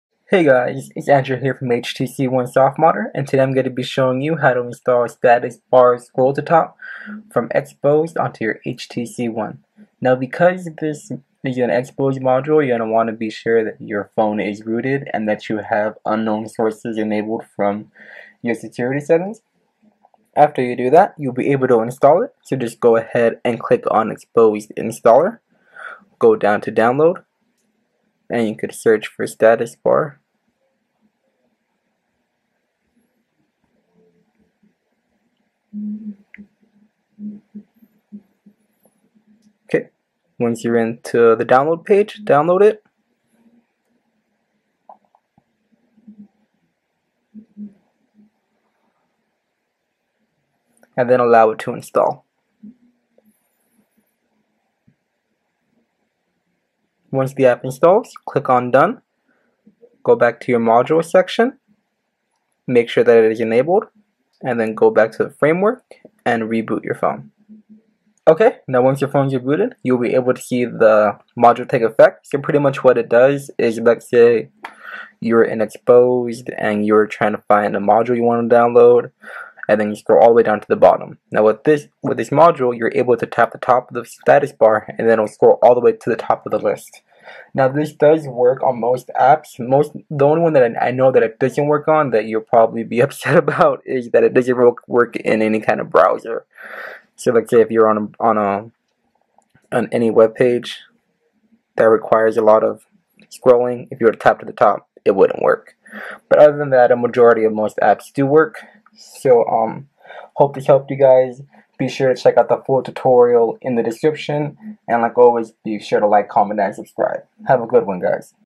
Instantly Scroll Back to the Top of Any App on Your HTC One [How-To]
How to Jump Back Up to the Top [HTC One]

In today's softModder guide, I'm going to show you how to install Statusbar Scroll to Top. Statusbar Scroll To Top is a Xposed module that allows you to tap the status bar to scroll to the top of an app, just like iOS devices. This doesn't work for all apps, but a majority to receive the functionality. You will need a rooted device with Xposed Framework installed.

For more information and more HTC One tips and tricks, check out the full article over on HTC One softModder using the link above.